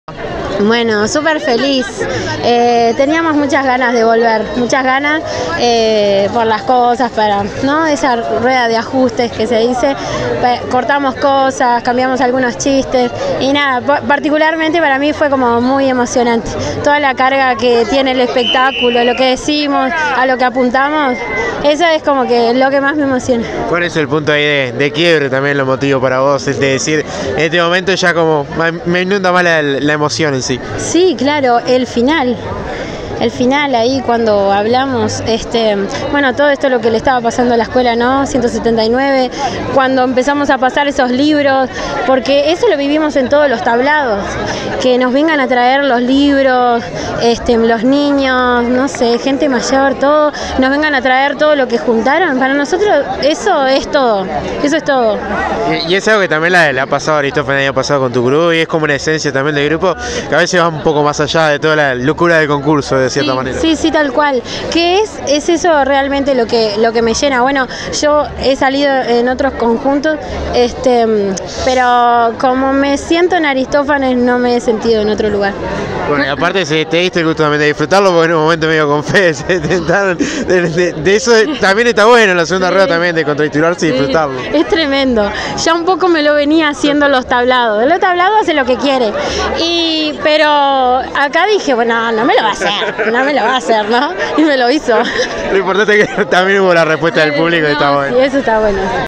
Camila Caeiro: “Fue muy emocionante toda la carga que tiene el espectáculo”
Aristophanes